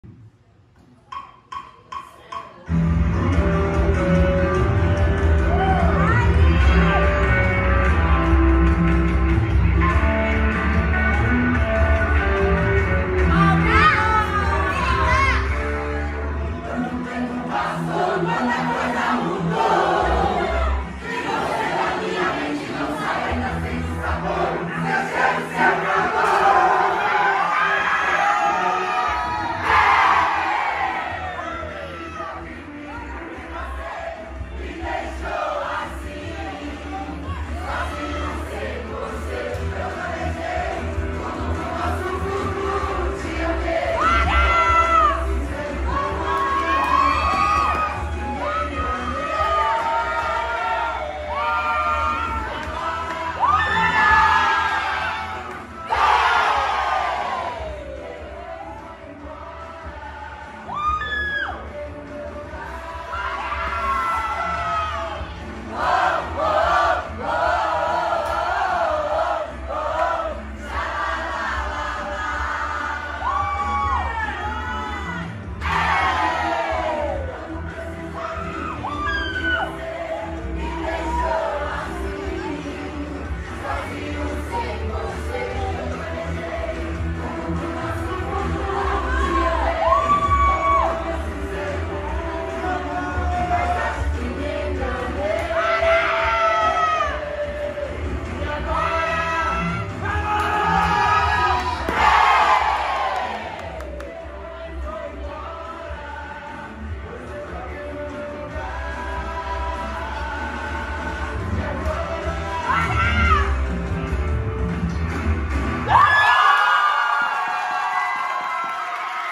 BRUNÃO MC E BRUNA RIO | APRESENTAÇAO 1 ANO DE ESPAÇO ATHUS
Um ano dessa escola linda, localizada em GRU, o espaço Athus dos incríveis Fagner Henrique e Núbia Mariana é uma das melhores escolas da zona norte/Guarulhos e conta com uma grade incrível de profissionais